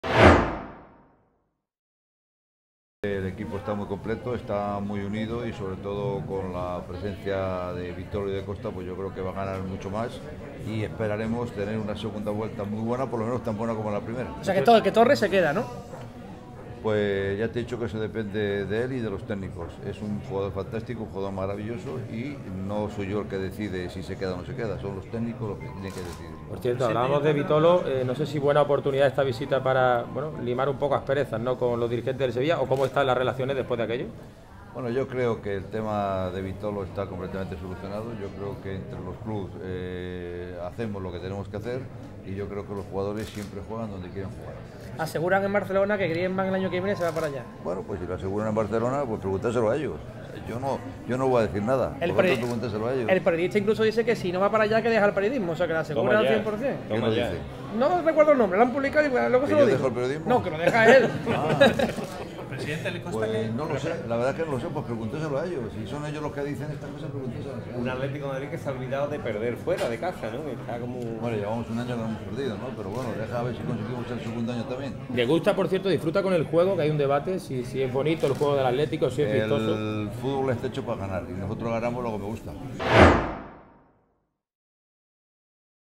Enrique Cerezo, en la Gala de ElDesmarque de Premios DEX
El presidente del Atlético de Madrid, Enrique Cerezo, atiende a los medios de comunicación en la Gala de ElDesmarque de Premios DEX.